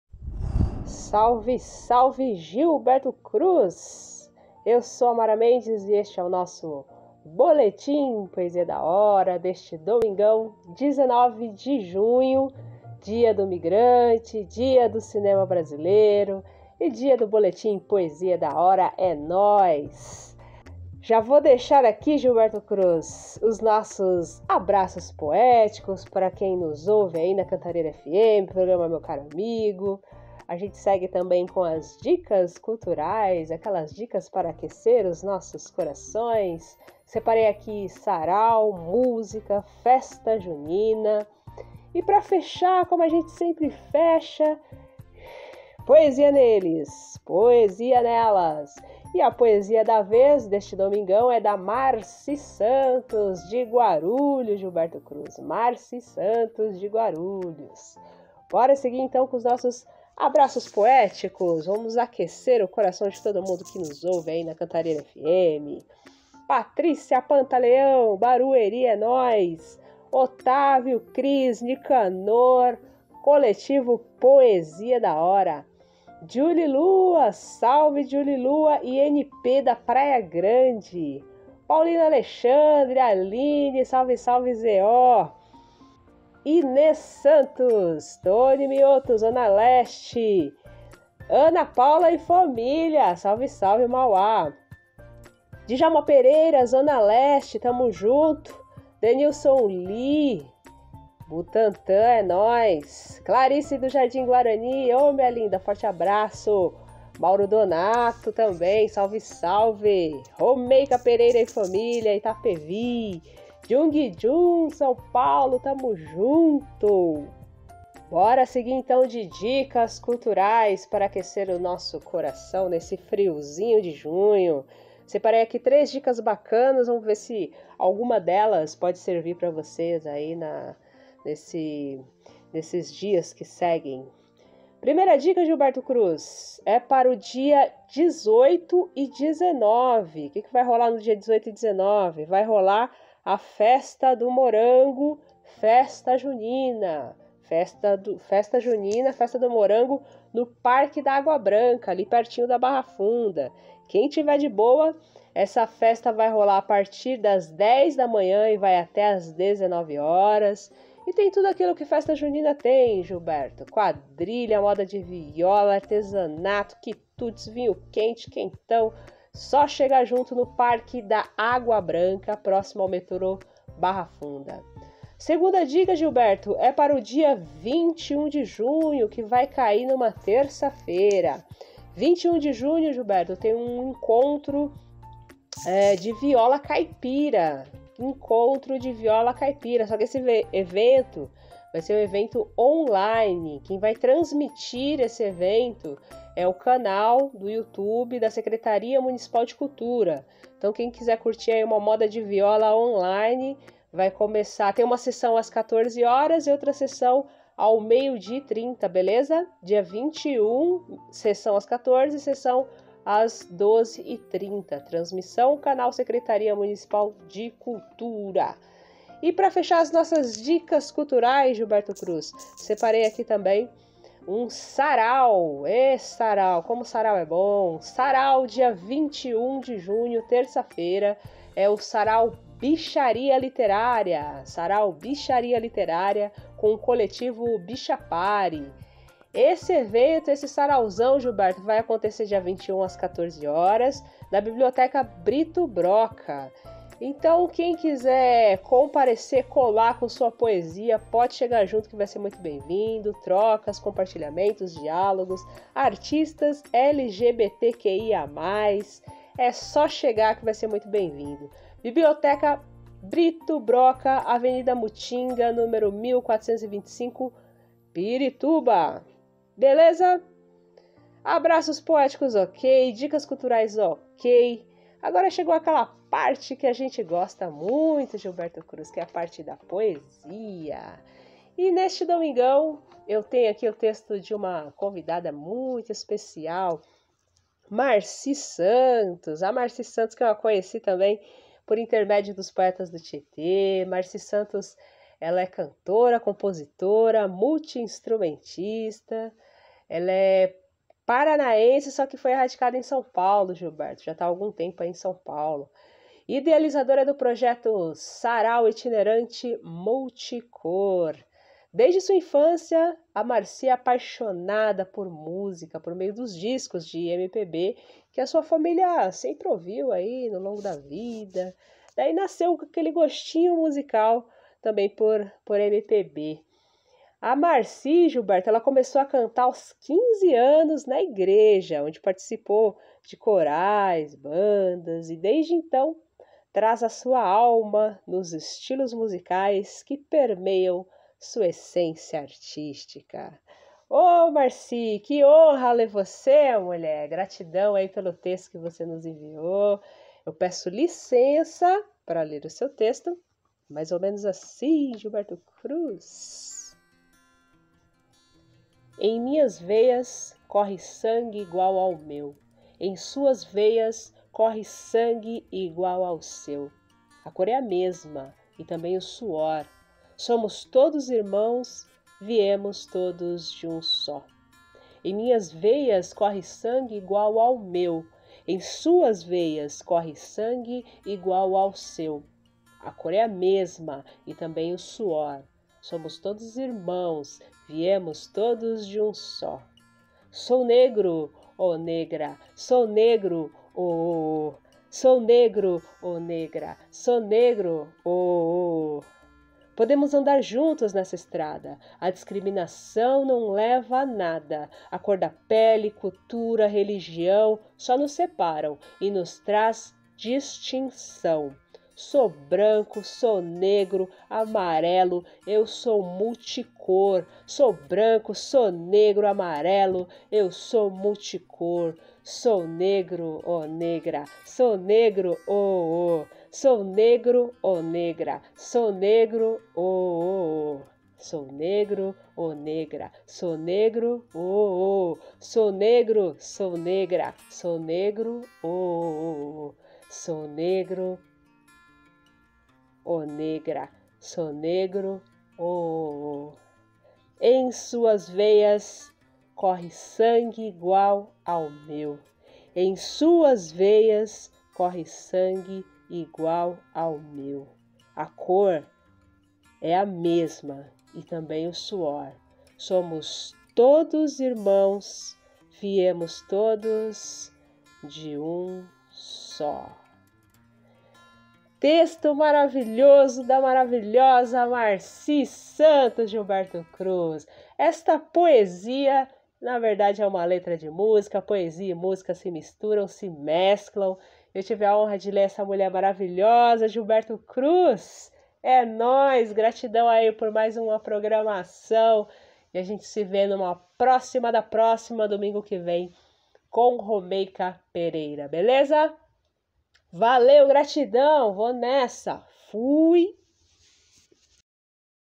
Boletim com texto de Marcy Santtos
Salveeeee.
Domingão vai rolar o Boletim Poesia é da hora com o texto da nossa querida Marcy Santos de Guarulhos. A Marcy é cantora, compositora, multiinstrumentista. Uma paranaense radicada em SP.
Desde a infância é apaixonada por música por meio dos discos de MPB que sua família ouvia. Dali também nasceu o seu gosto musical.
Começou a cantar aos 15 anos na igreja onde participou dos corais e bandas.
Desde então traz sua alma nos estilos musicais que permeiam sua essência artística

Para acompanhar:
19/06 às 10h40
Cantareira FM, programa Meu Caro Amigo.
Canal Poesia é da hora, YouTube.